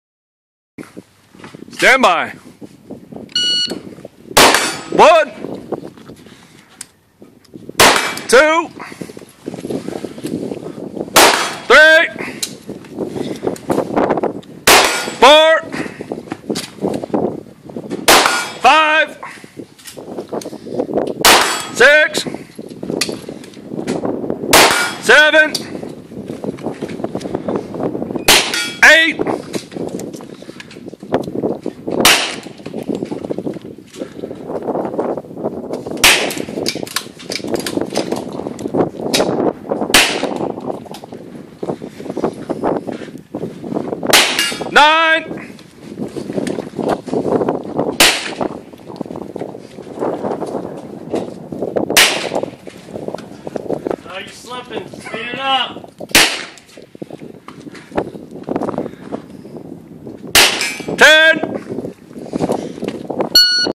60 Seconds
How many hits can you get in 60 seconds? Rifle starts with 7 rounds. Pistol fully loaded. One attempted shot from each shooting position.